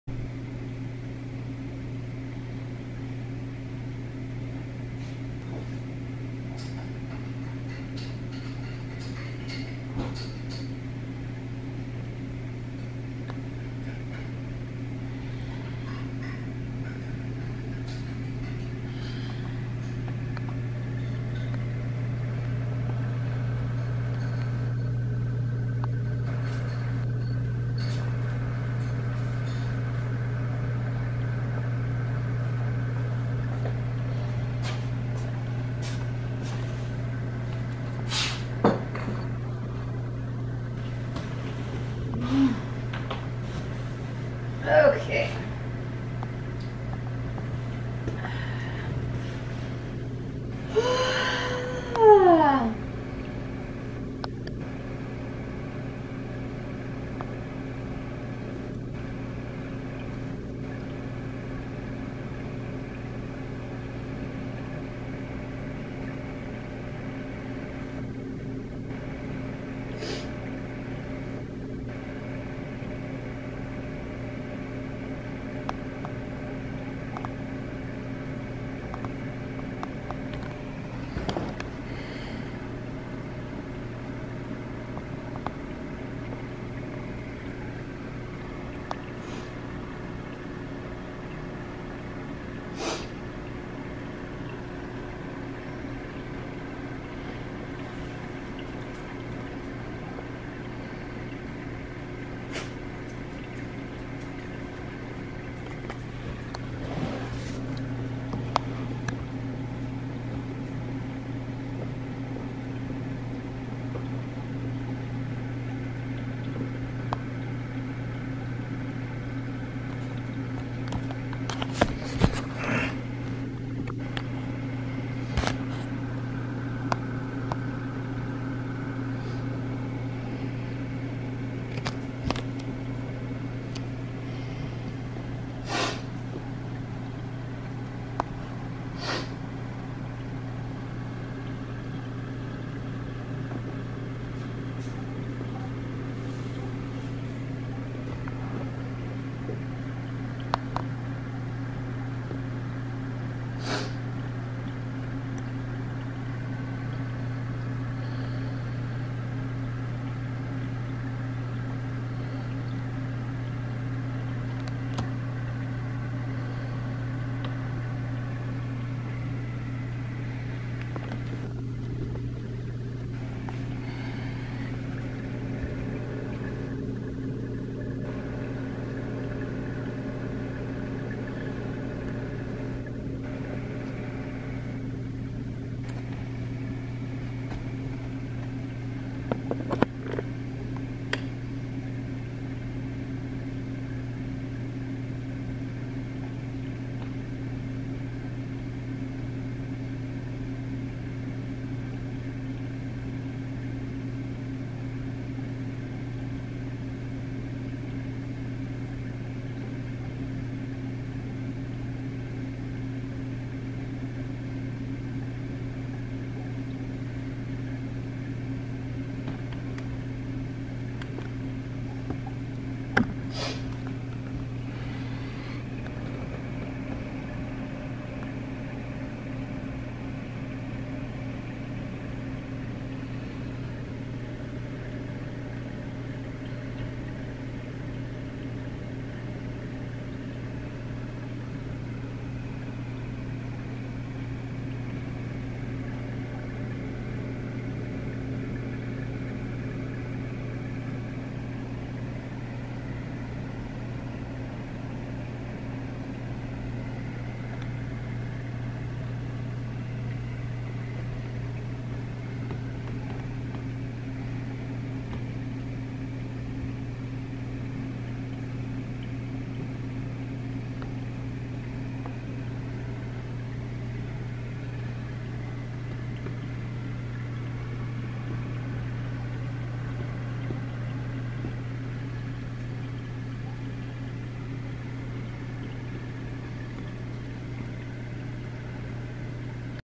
New corals new fish and feeding live baby brine shrimp
2 new dispar anthias, 1 new lyre tail anthia, 1 Green Mandrin Gobie. New favia and  monti corals. LOTS of hair algae. . . . . . and a tuxedo urchin to help with that problem.